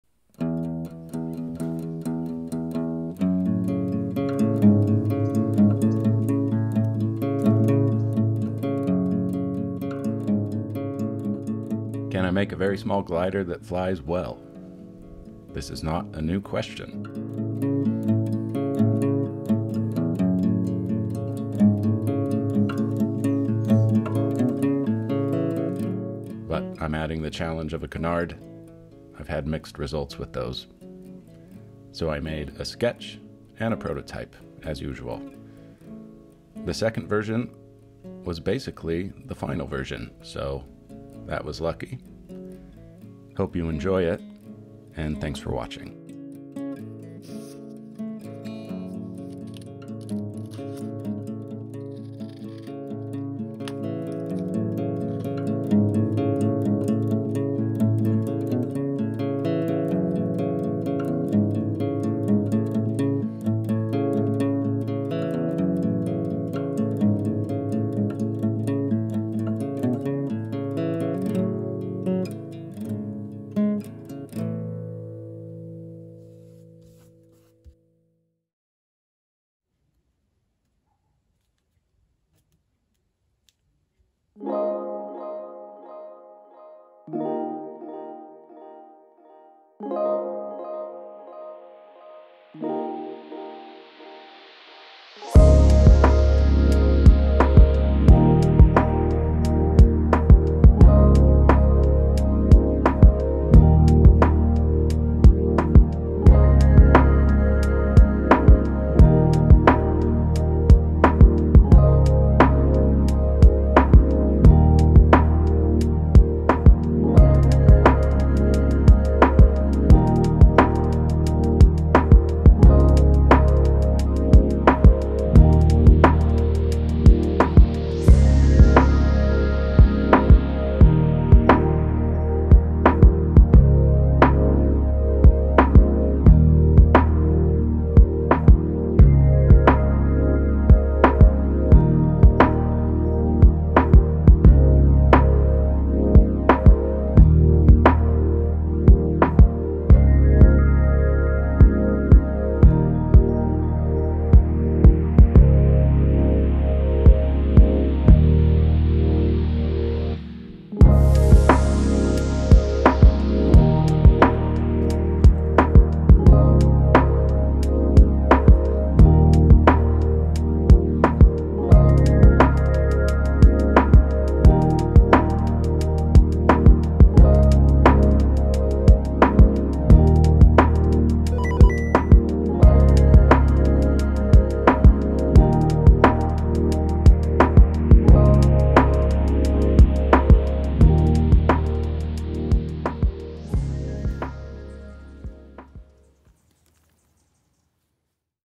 Micro Canard Glider Made of Card Stock (episode 45)
Original canard glider design! Get a beige file folder or sheet of 80 lb. / 215 gsm card stock, as well as scissors, a ruler, and a hobby blade. Make your own micro canard glider in under an hour.

Wingspan of 11cm; fuselage length of 8.4cm.

Plans and instructions for sale: